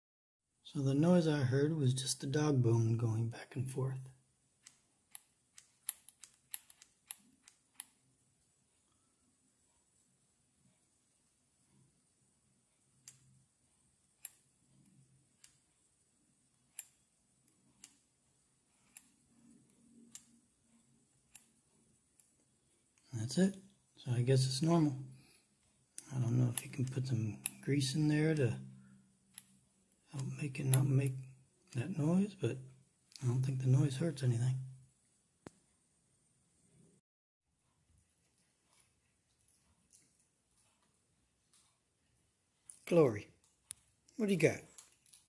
Losi Lmt Build _3 -(Axle noise was the dog bone sliding around)
Turns out the noise I heard from the back axle was the dog bone sliding around. Appears to be normal as far as I can tell.

Very happy to see my roll has the new V2 axles that are beefed up with cross hatching.